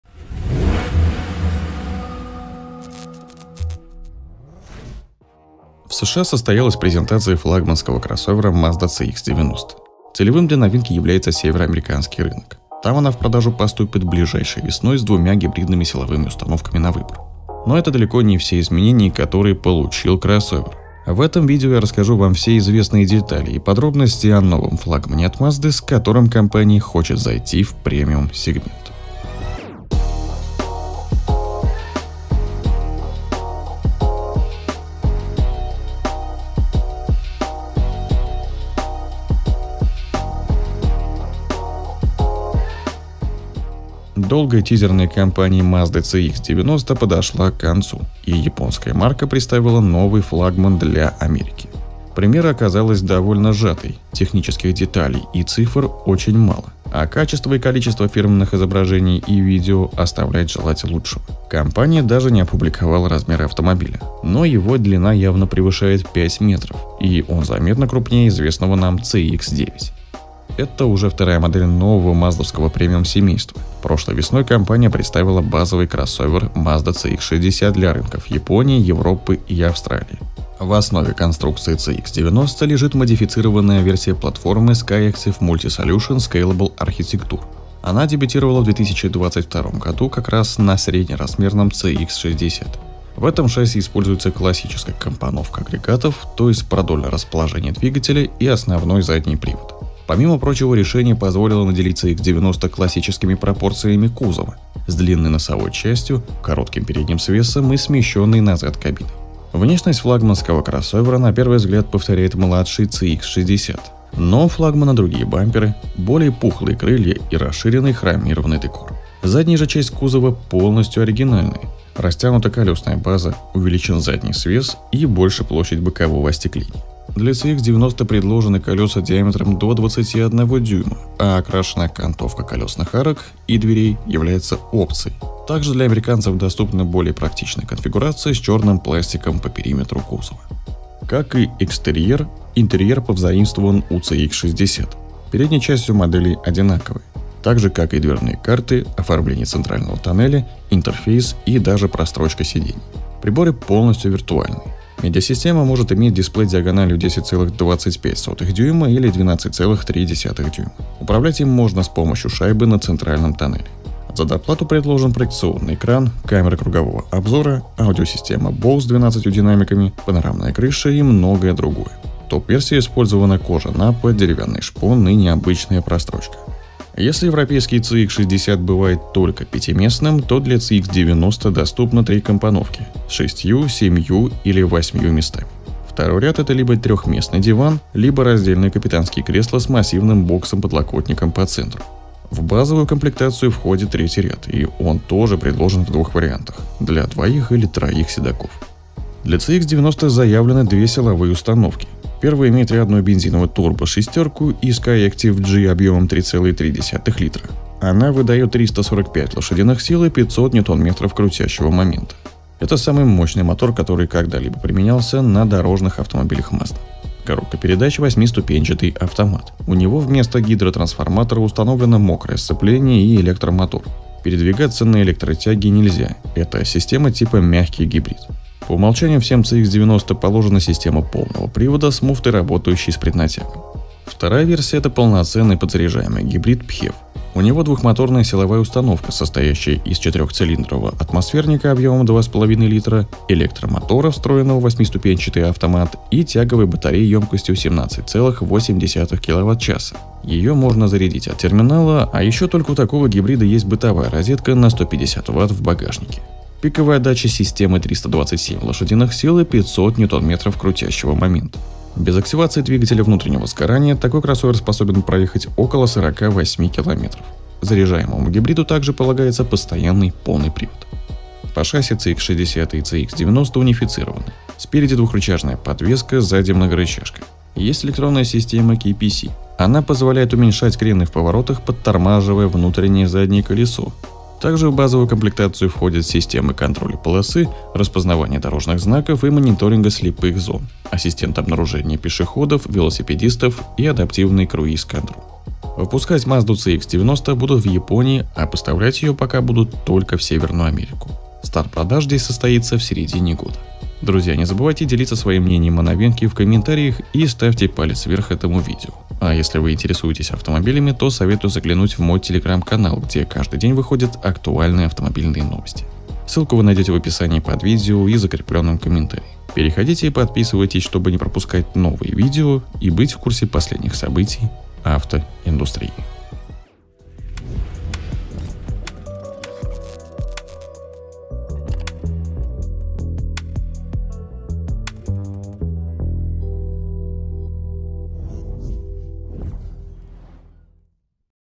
Роскошная MAZDA CX-90 | Cамый большой и мощный SUV от Мазды
В США состоялась презентация флагманского кроссовера Mazda CX-90. Целевым для новинки является Североамериканский рынок, в продажу она поступит ближайшей весной с двумя гибридными силовыми установками на выбор.